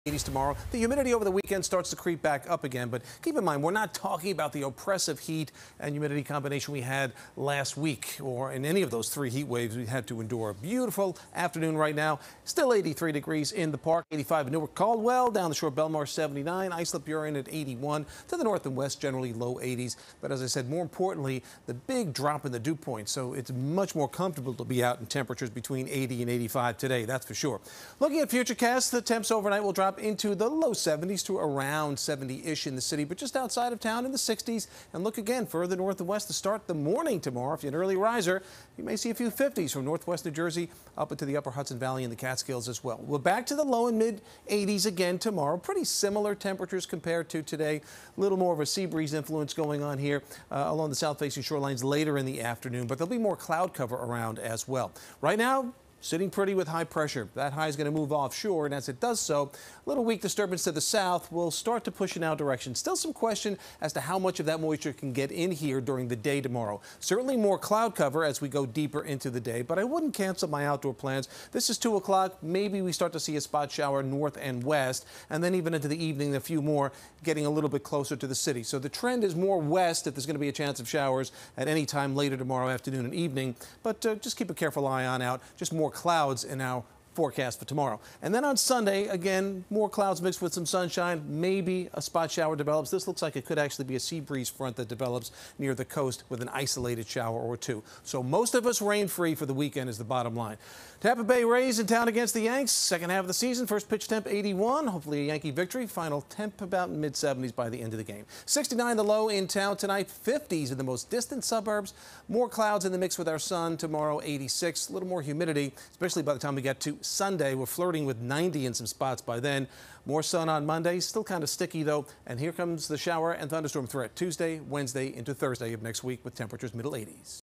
Summer humidity returns to NYC; extreme heat stays away in the forecast
The week ends on a great note, but the summer humidity will be making a return. While it will be on the warm side, the extreme heat will be lacking heading into next week.